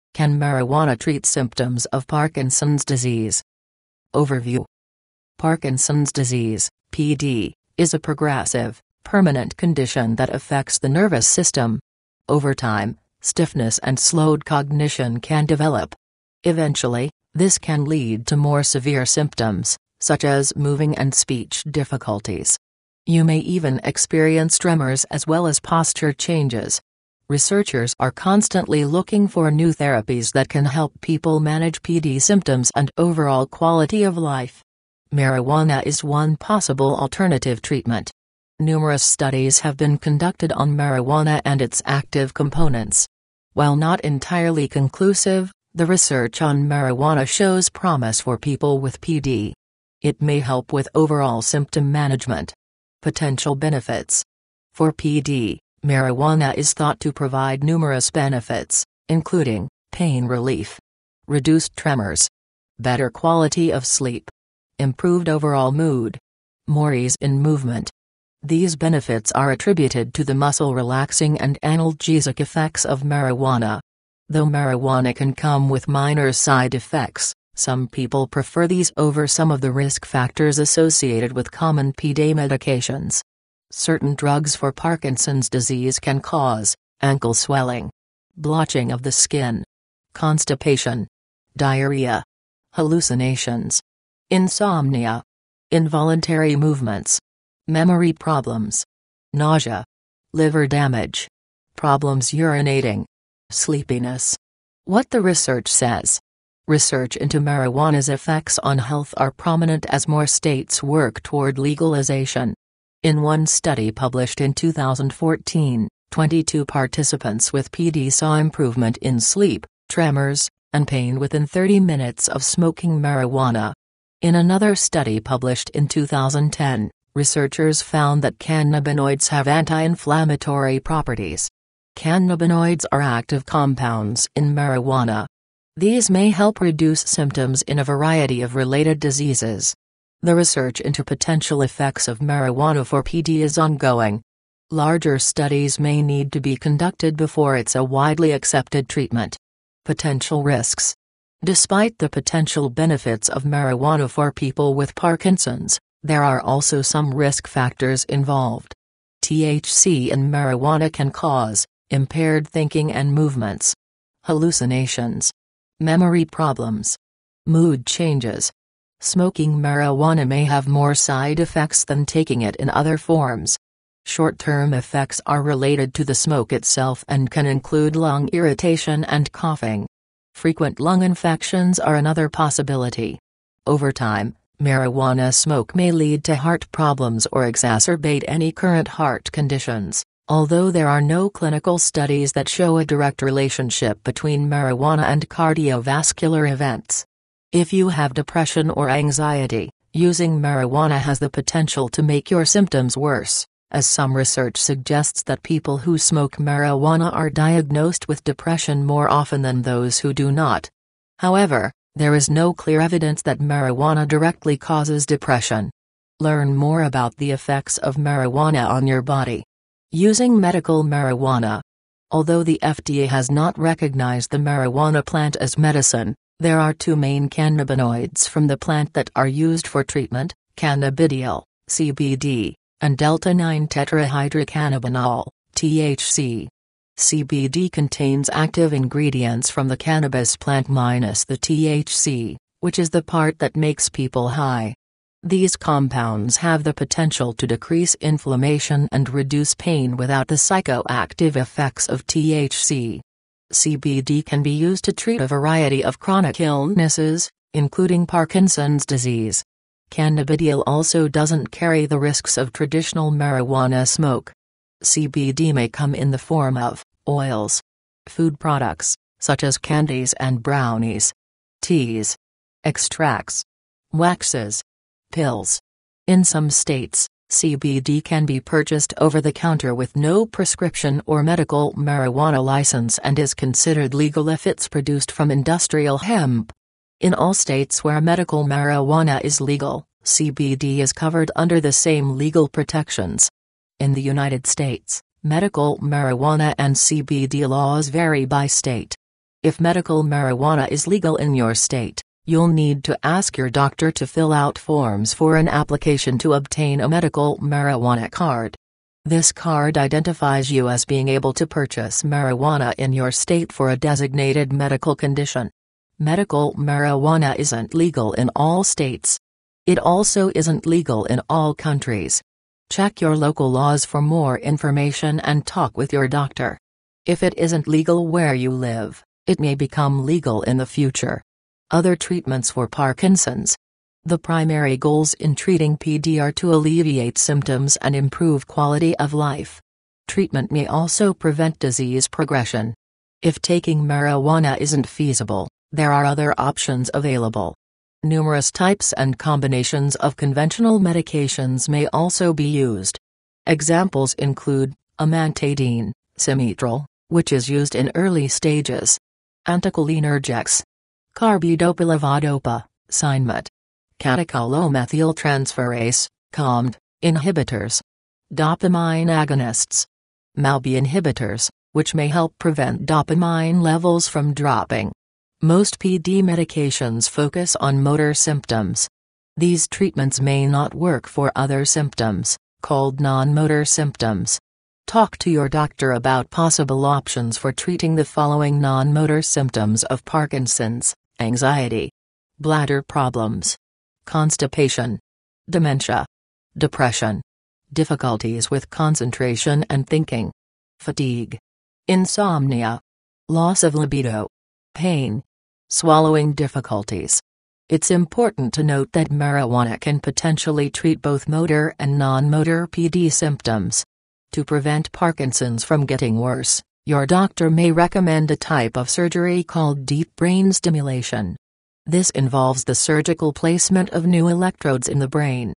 Can Marijuana Treat Symptoms of Parkinson’s Disease
Would marijuana be able to Treat Symptoms of Parkinson's Disease.

Outline.

Parkinson's sickness (PD) is a dynamic, lasting condition that influences the sensory system. After some time, solidness and hindered cognizance can create. Inevitably, this can prompt more serious manifestations, for example, moving and discourse challenges. You may even experience tremors and also pose changes.

Analysts are continually searching for new treatments that can enable individuals to oversee PD side effects and general personal satisfaction. Weed is one conceivable elective treatment.

Various investigations have been led on pot and its dynamic segments. While not by any stretch of the imagination definitive, the exploration on weed indicates guarantee for individuals with PD. It might help with general manifestation administration.

Potential advantages.

For PD, pot is thought to give various advantages, including:

relief from discomfort.

diminished tremors.

better nature of rest.

enhanced general disposition.

more straightforwardness in development.

These advantages are credited to the muscle-unwinding and pain relieving impacts of maryjane.

In spite of the fact that weed can accompany minor symptoms, a few people incline toward these over a portion of the hazard factors related with normal PD solutions. Certain medications for Parkinson's malady can cause:

*ankle swelling.

*blotching of the skin.

*constipation.

*diarrhea.

*hallucinations.

*insomnia.

*involuntary developments.

*memory issues.

*nausea.

*liver harm.

*problems urinating.

*sleepiness.

What the exploration says.

Research into weed's impacts on wellbeing are unmistakable as more states progress in the direction of sanctioning. In one examination distributed in 2014, 22 members with PD saw change in rest, tremors, and torment inside 30 minutes of smoking weed.

In another investigation distributed in 2010, specialists found that cannabinoids have mitigating properties. Cannabinoids are dynamic mixes in cannabis. These may help diminish manifestations in an assortment of related ailments.

The examination into potential impacts of maryjane for PD is progressing. Bigger examinations may should be directed before it's a generally acknowledged treatment.

Potential dangers.

Regardless of the potential advantages of weed for individuals with Parkinson's, there are additionally some hazard factors included. THC in maryjane can cause:

*impaired considering and developments.

*hallucinations.

*memory issues.

*mood changes.

Smoking pot may have more reactions than taking it in different structures. Here and now impacts are identified with the smoke itself and can incorporate lung bothering and hacking. Visit lung contaminations are another probability. After some time, maryjane smoke may prompt heart issues or compound any present heart conditions, despite the fact that there are no clinical examinations that demonstrate an immediate connection amongst cannabis and

In a few states, CBD can be obtained over the counter with no solution or medicinal cannabis permit and is viewed as legitimate if it's delivered from modern hemp. In all states where restorative maryjane is legitimate, CBD is secured under the same lawful assurances.